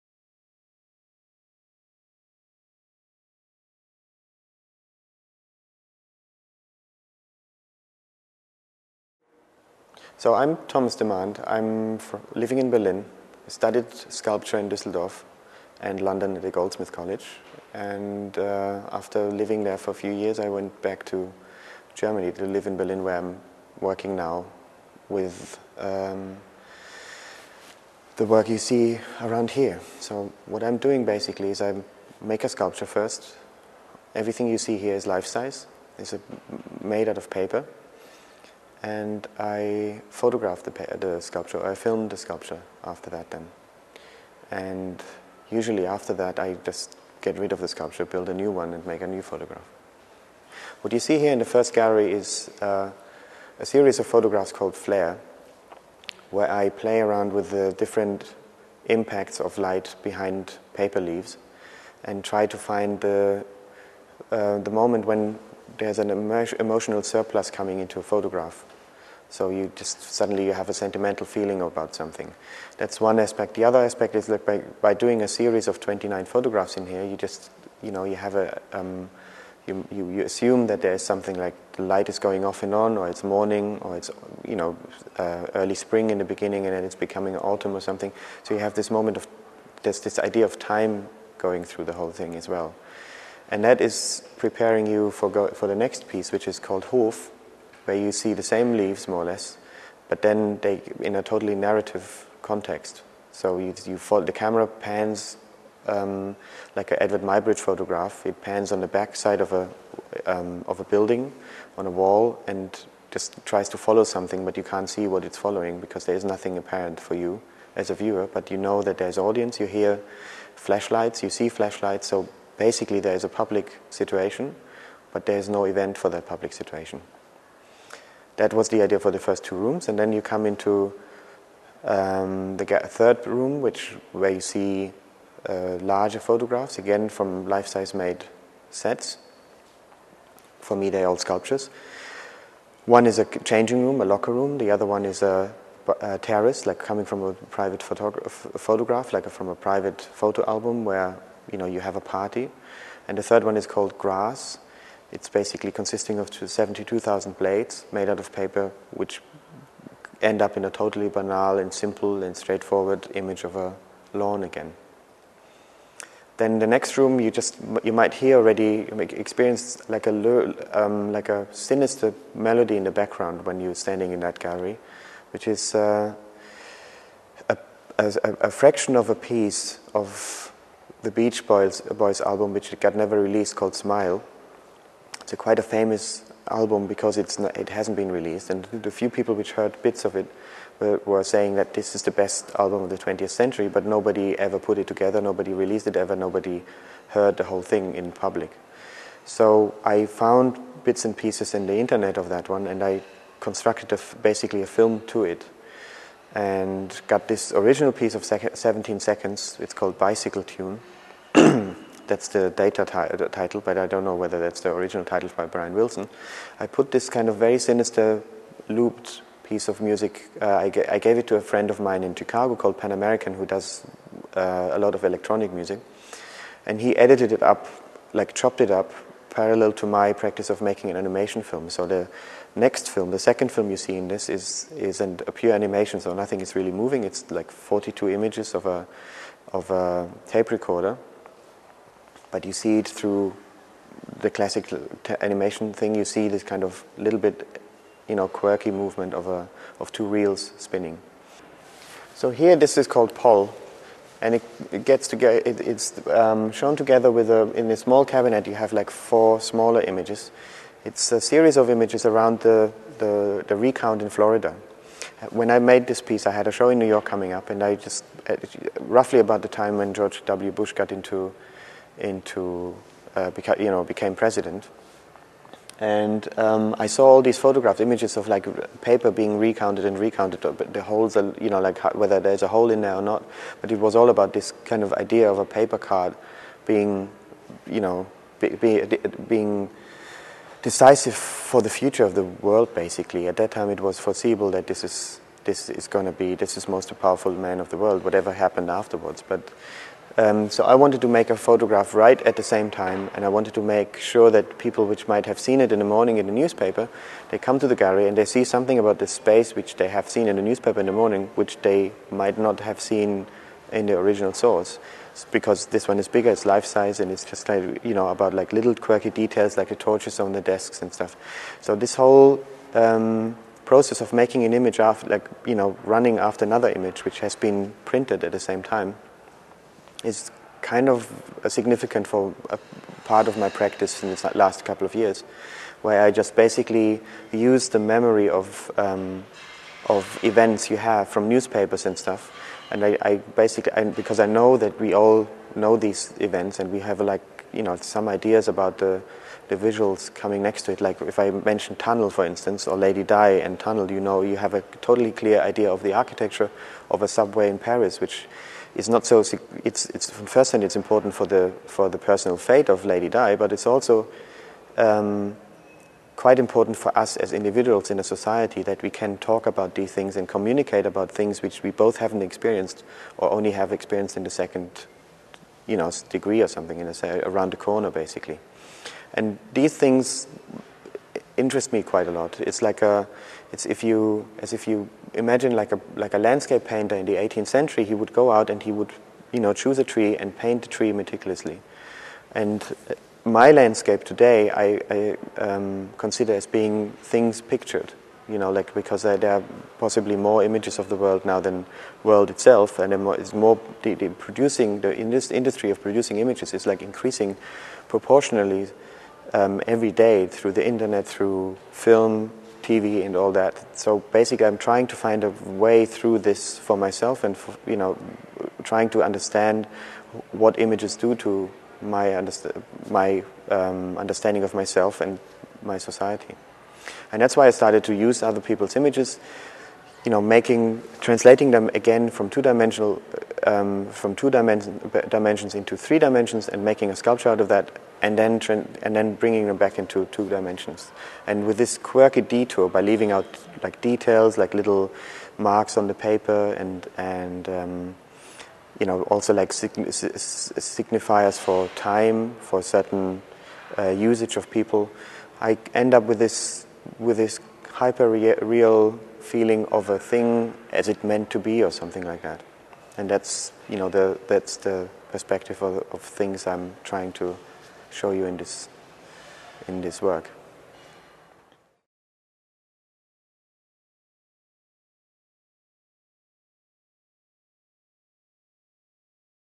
Thomas Demand, artist talk at DCA.flv
Artist Thomas Demand talking about his exhibition (21 June - 10 August 2003) at Dundee Contemporary Arts.